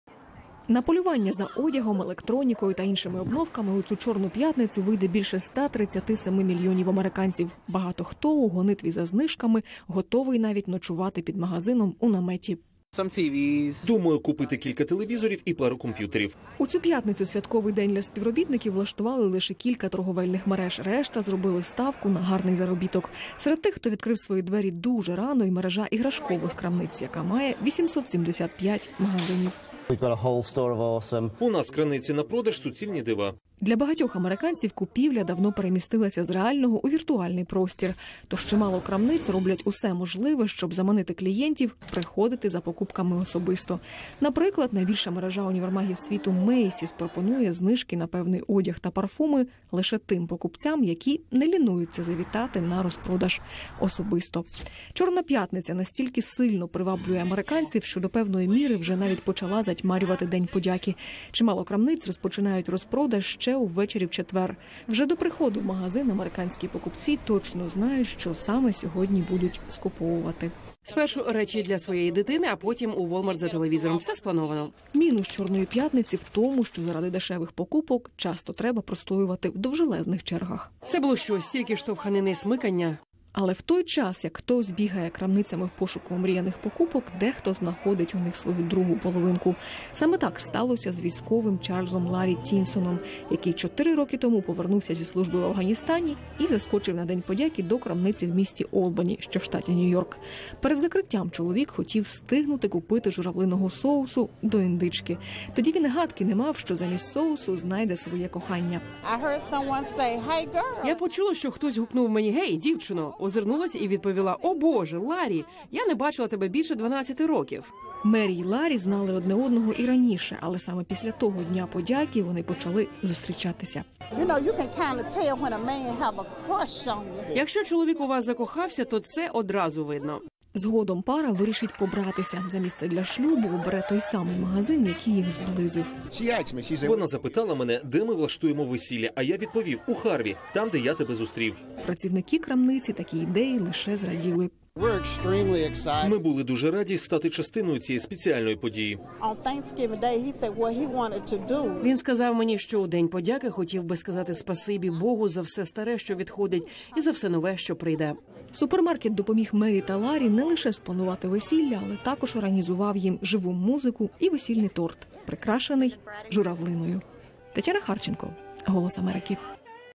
Розпродажі в США до Дня подяки
«Чорна п’ятниця» - американський марафон покупок